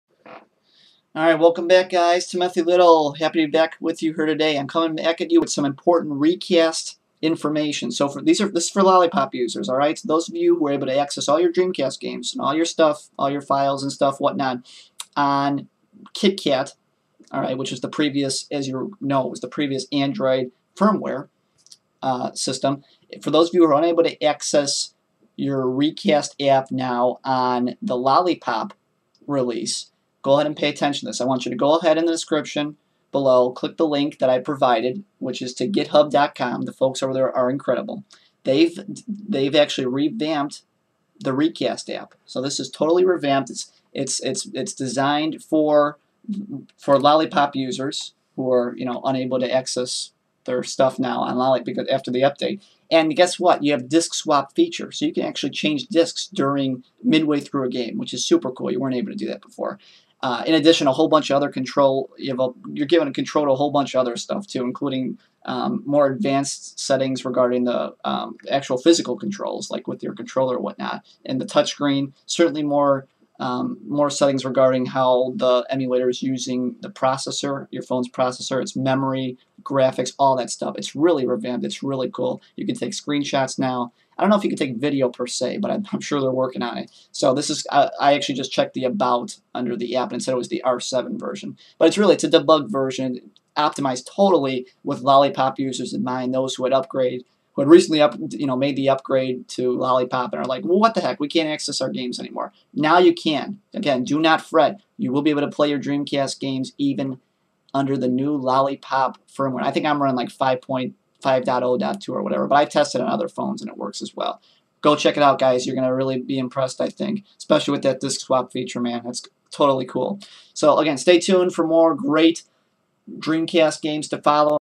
Latest Reicast Build for Android Lollipop
Hey everyone, check the link below to the new and improved  Reicast app. Latest build designed to work with Android Lollipop. The latest version features disc swap among other awesome updates! Go check it out!